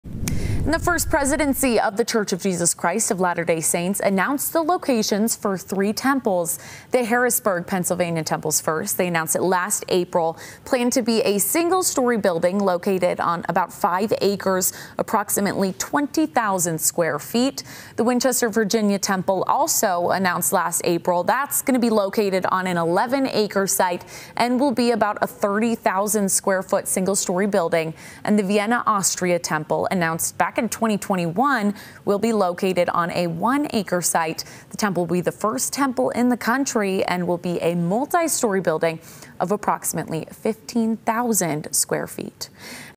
Three new temple sites announced: Austria, Pennsylvania, Virginia
The First Presidency of The Church of Jesus Christ of Latter-day Saints announced the locations for the Vienna Austria Temple, Harrisburg Pennsylvania Temple, and the Winchester Virginia Temple.